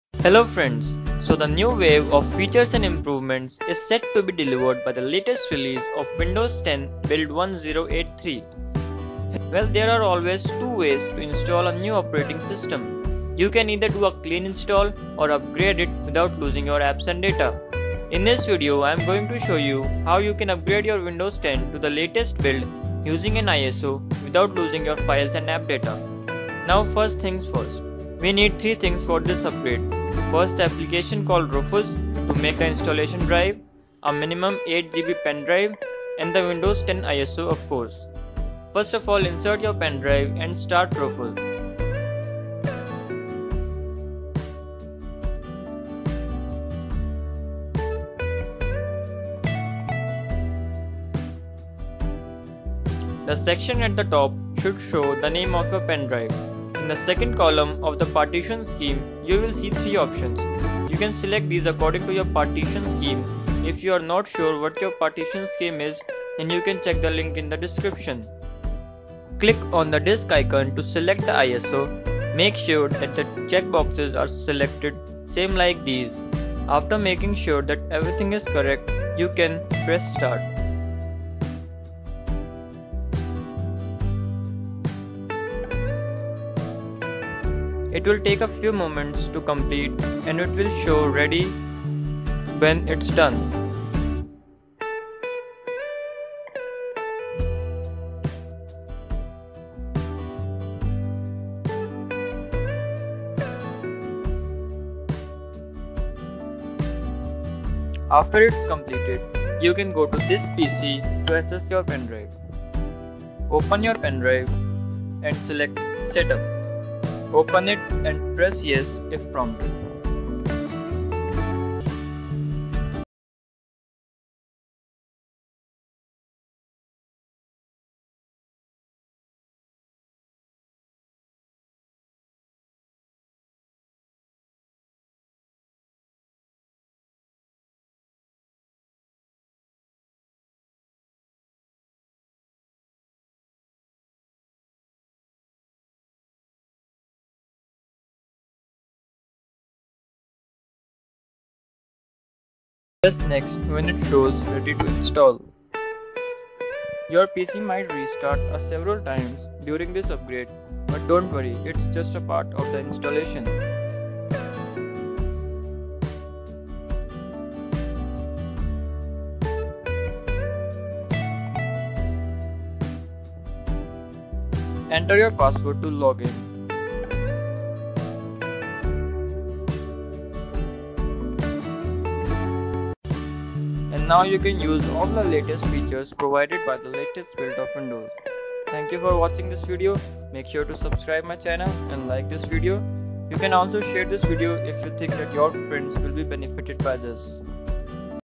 How to install or Update Windows 10 1803 using ISO file
Update windows to latest windows 10 1803 using iso file.